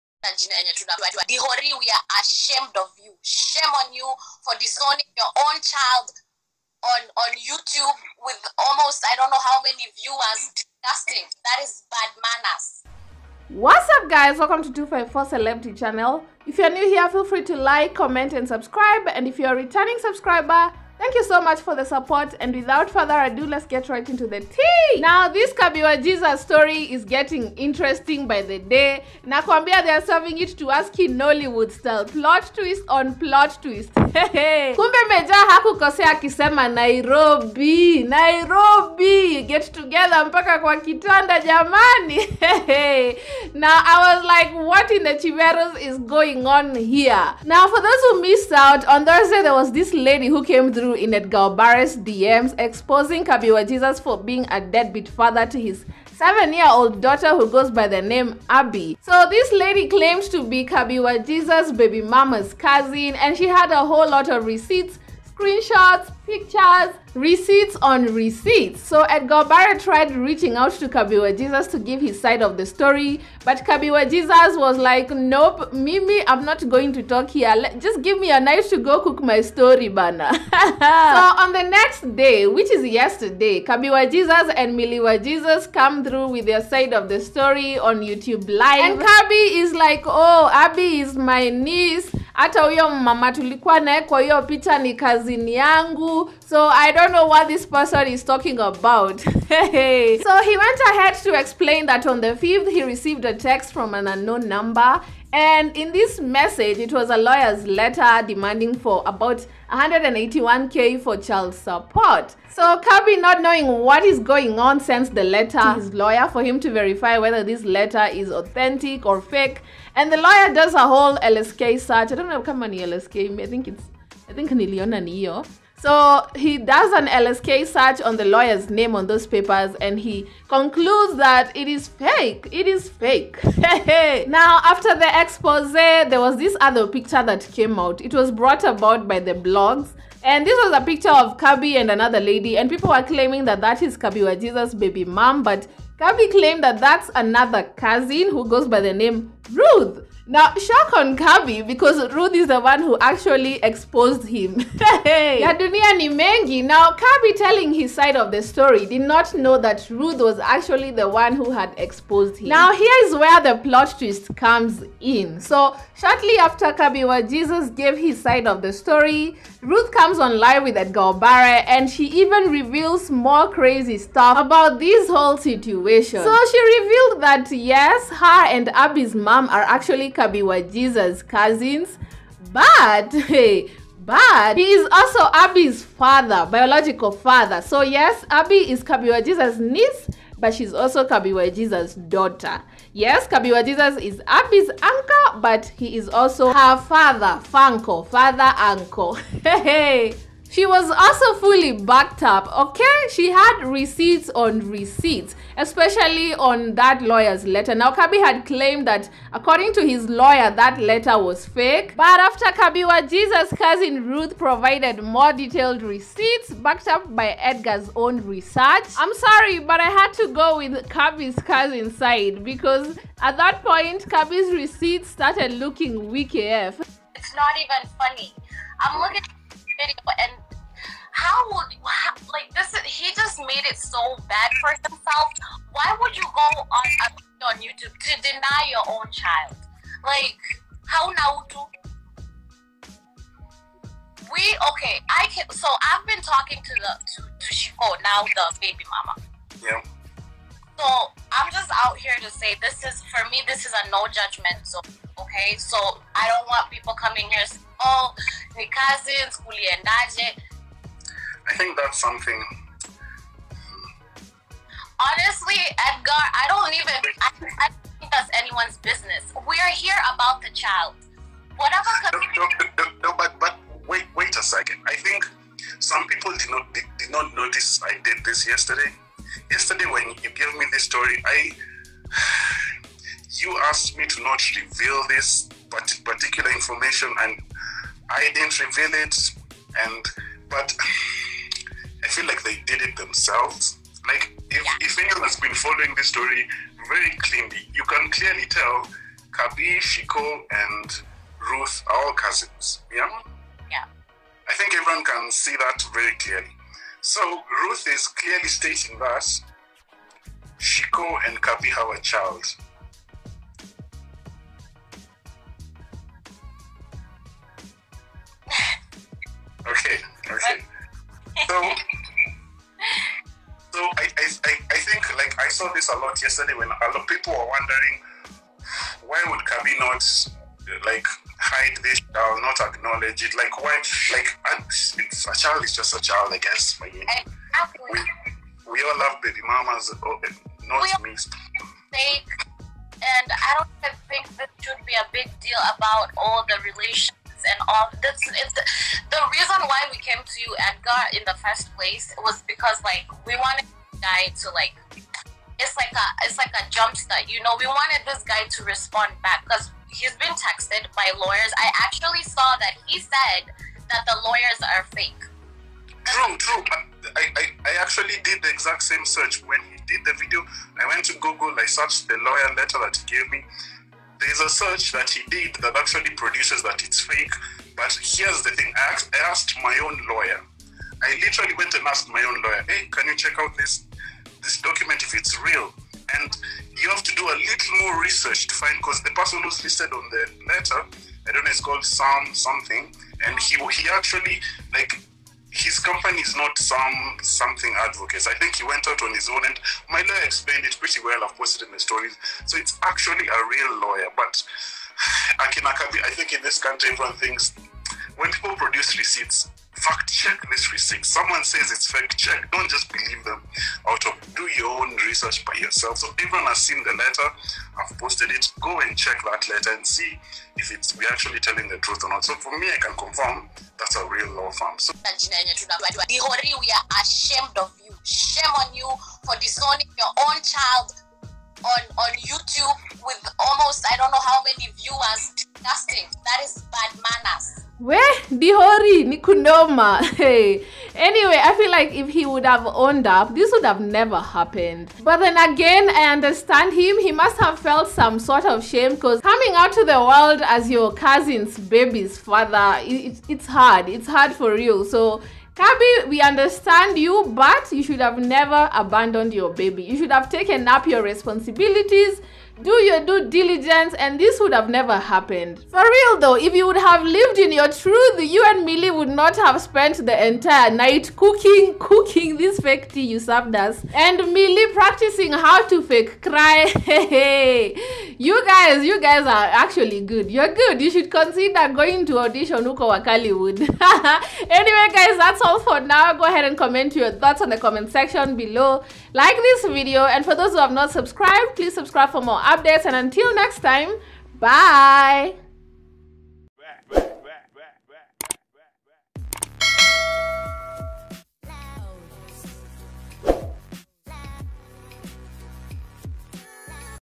DRAMA! KABI WA JESUS EXPOSED AS HIS COUSIN'S BABY FATHER AFTER DENYING HIS DAUGHTER ON LIVE
Kabi Wa Jesus cousin Ruth came through to expose Kabi as his other cousin's baby father shortly after doing a live video denying the child.